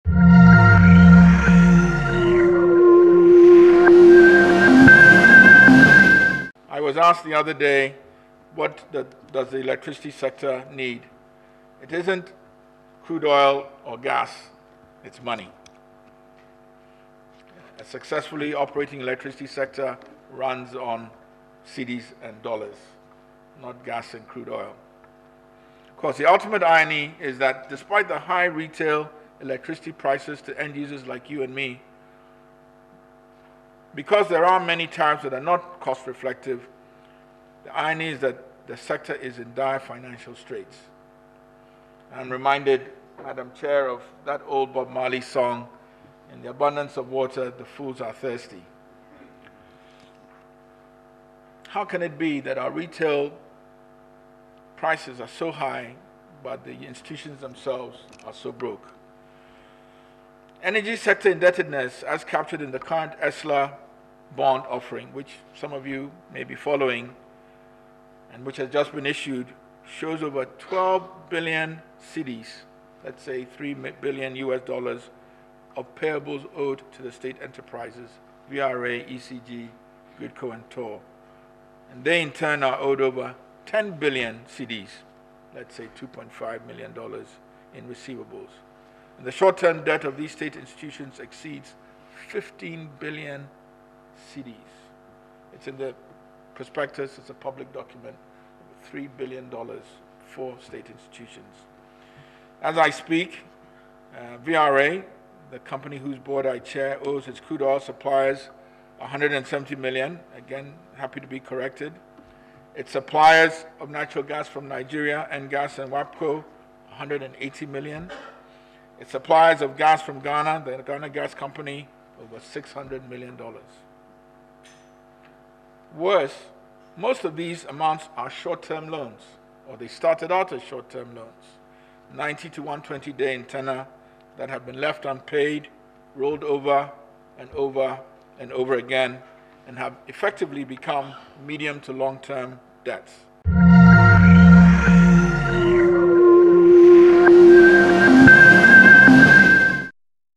Debts crippling energy sector – VRA Board Chairman
Board Chairman of the VRA, Kweku Andoh Awotwi says the energy sector is choked with debts.